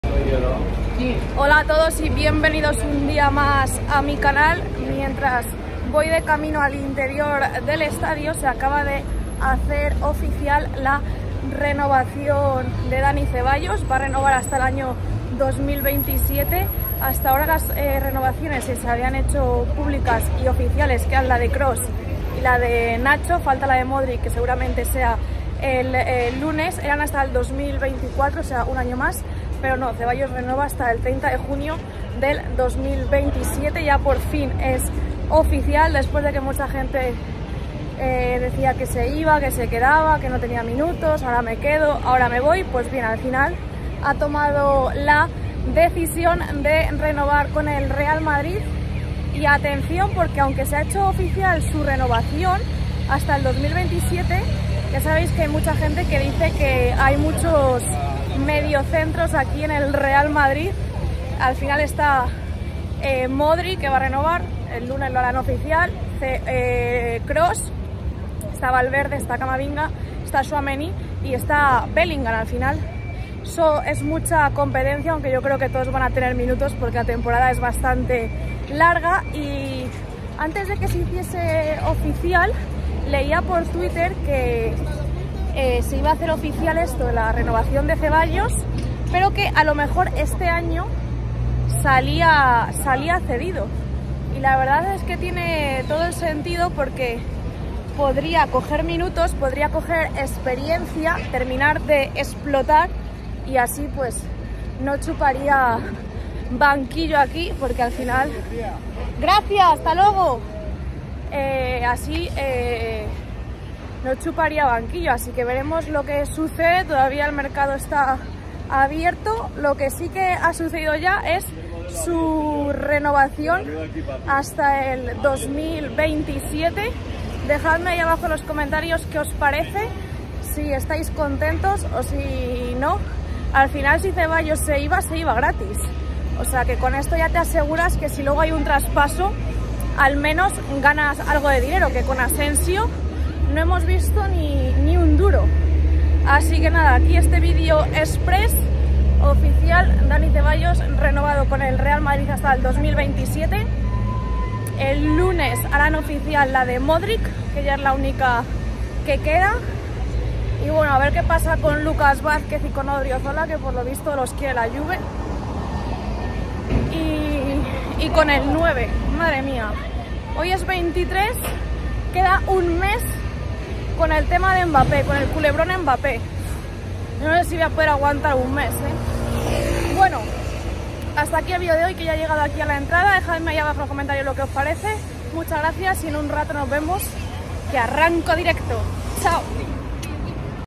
🚨 OFICIAL: DANI CEBALLOS RENUEVA CON EL REAL MADRID
🚨 Equipo de producción que utilizo para mis videos
🔴 GRABO con un SAMSUNG S22 ULTRA 5G

🫂 SOMOS un EQUIPO de 88.210 PERSONAS